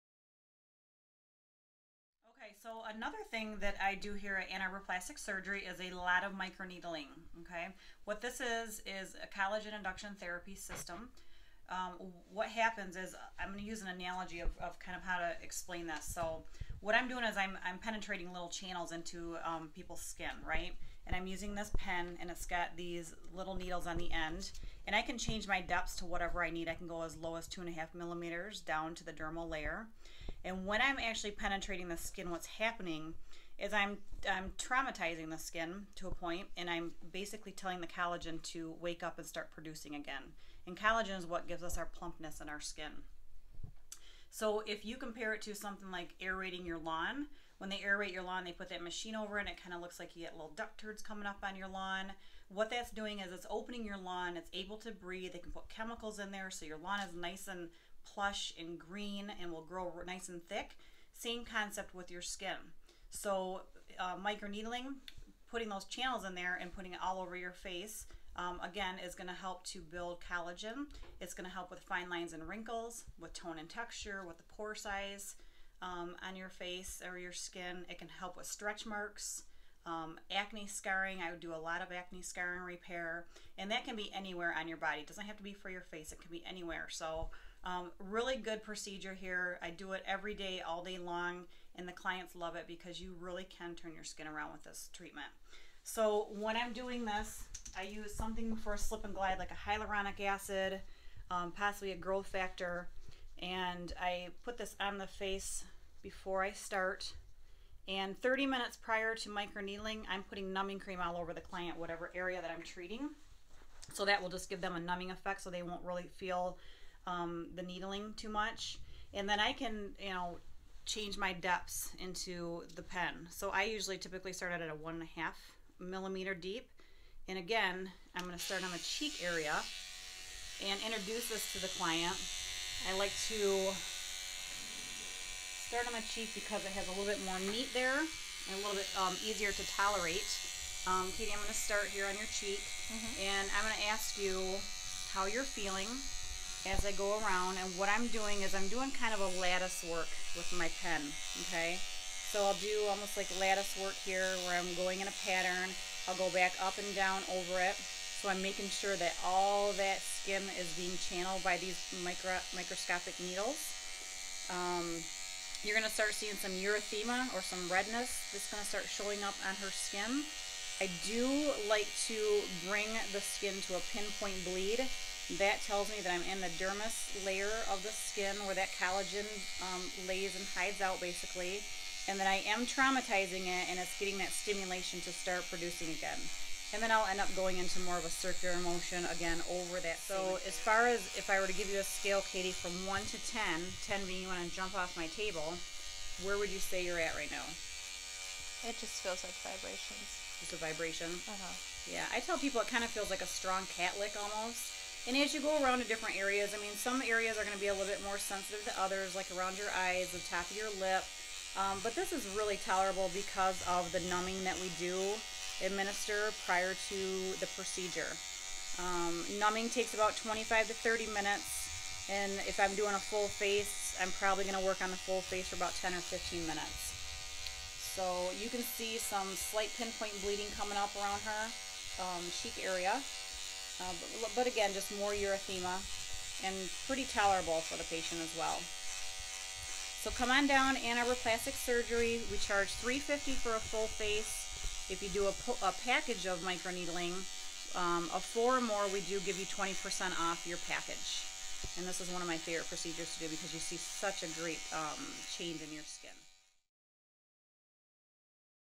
Microneedling, a Collagen Induction Therapy
Watch as Lisa, Medical Aesthetician discusses and demonstrates Microneedling, a Collagen Induction Therapy.